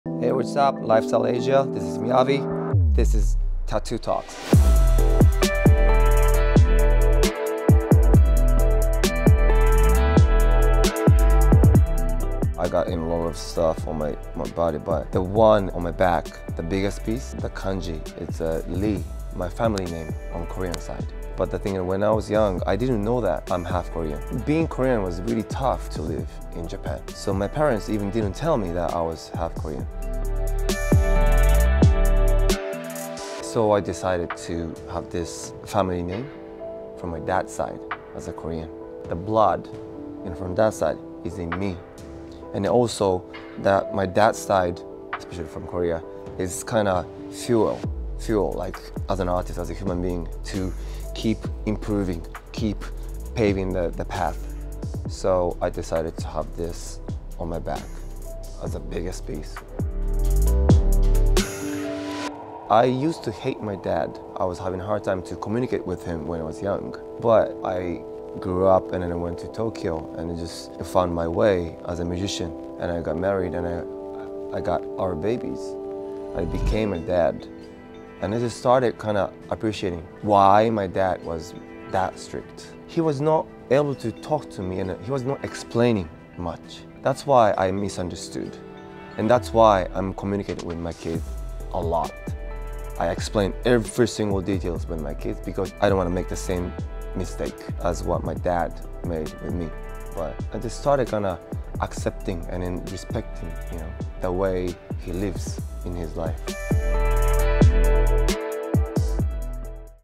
Tattoo Talks: Miyavi
n our next episode of ‘Tattoo Talks,’ we join MIYAVI over a glass of The Macallan at Carlyle and Co. MIYAVI’s tattoos reflect how he lives his life — full of purpose and emotion.

The tattoo he goes over with us was the largest on his body. Located on his back and created by Kotaro Sato, the character “李” can be found enveloped by the Heart Sutra. Many might think that this indicates the character of the Japanese plum “sumono” but it is actually for his Korean last name “Lee.” Growing up half-Korean in Japan, MIYAVI was sheltered from knowing his ancestry at a young age due to the stigma surrounding his ethnicity in Japan. Coupled with the fact that MIYAVI grew up butting heads with his father, which distanced the two, there was no reason for embracing his father’s side... that is until he became a father himself.

Recognising that his father’s blood flows through his veins as well, MIYAVI eventually took a new approach in trying to understand his actions and used this journey as fuel for his artistry. While creating a family for himself, he finally understood the source of his frustration with his father and the lack of communication and expression. While respect for his father’s way of life was eventually established, MIYAVI reveals that through this exploration of his heritage, he learned not to repeat the same mistakes and to raise his children so that there is never any misunderstanding.

Video Credits:
Video: Ryan Putranto
Editor: Samson De Guzman
Producer: Ambrose Leung